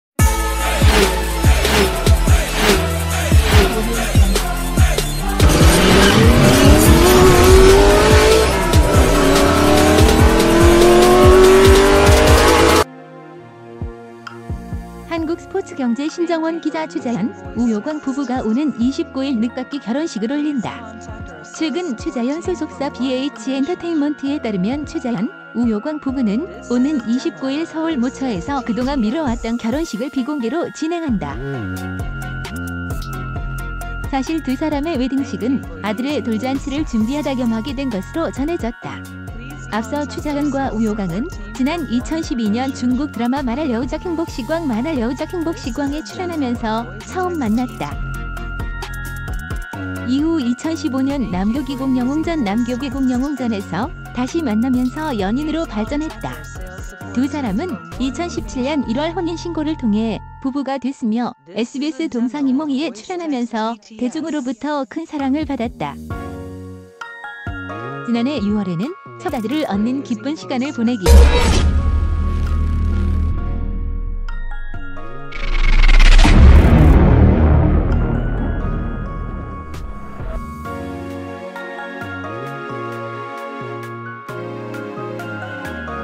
추자현♥우효광, 오는 29일 늦깎이 웨딩마치 '아들 돌잔치도 함께'
,추자현♥우효광,#,#'아들,오는,돌잔치도,29일,추자현♥우효광,#,#'아들,
추자현♥우효광, 오는 29일 늦깎이 웨딩마치 '아들 돌잔치도 함께' ► Subscribe for more [한국스포츠경제=신정원 기자]추자현, 우효광 부부가 오는 29일 늦깎이 결혼식을 올린다. 최근 추자현소속사 BH엔터테인먼트에 따르면 추자현, 우효광 부부는 오는 29일 서울 모처